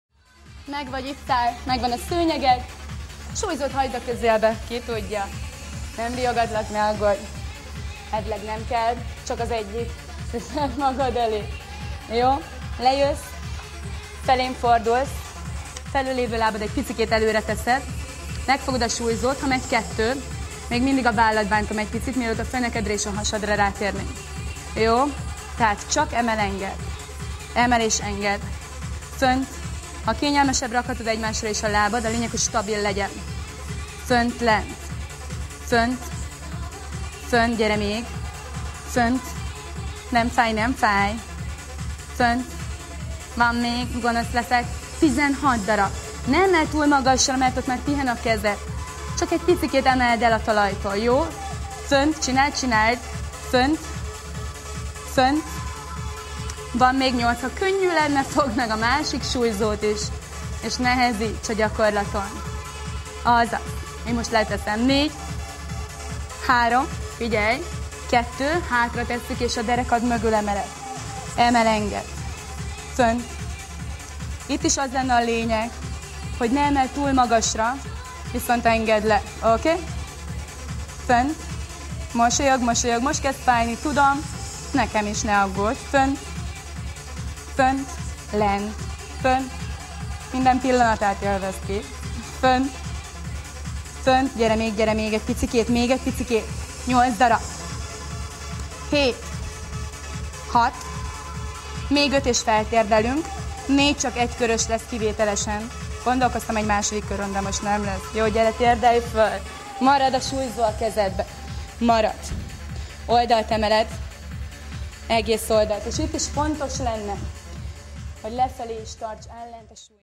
Kondicionálás Szalka Andival
Ha jó edzettségi állapotban vagy, ne hagyd ki Andi óráját, mert megmutatja Neked,milyen is egy igazi kondicionáló óra.